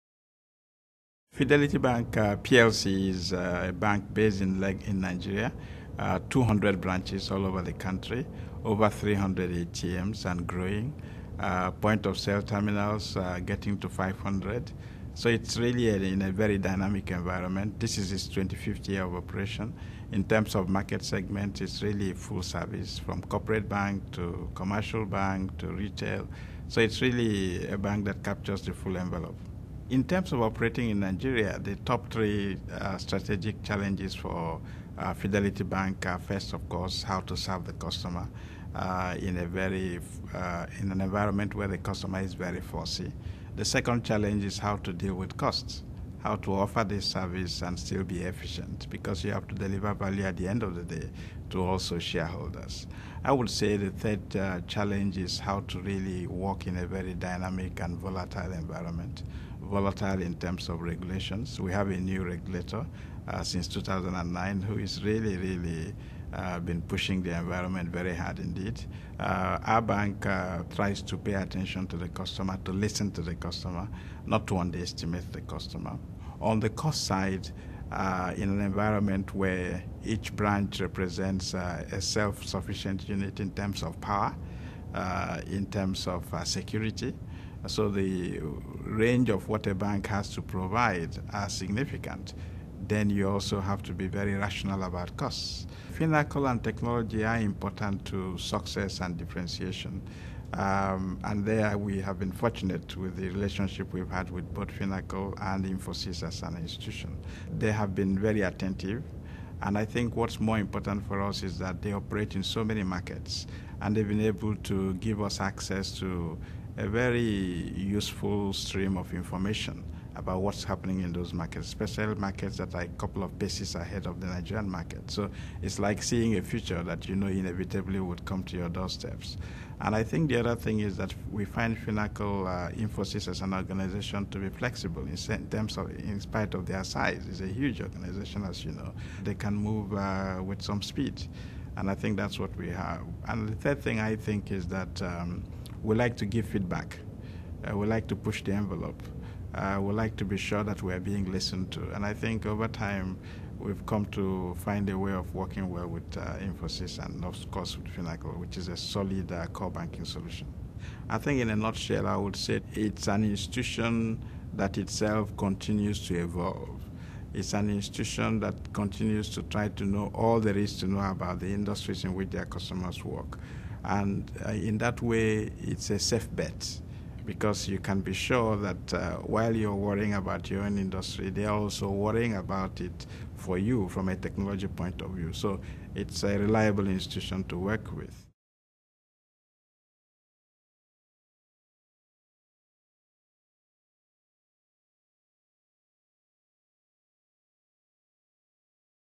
Finacle - Reginald Ihejiahi M.D & Chief Executive Fidelity Bank Plc
Reginald Ihejiahi M.D. & Chief Executive Fidelity Bank Plc discusses importance of technology and how Finacle has helped Fidelity Bank drive business growth and acquire new customer base.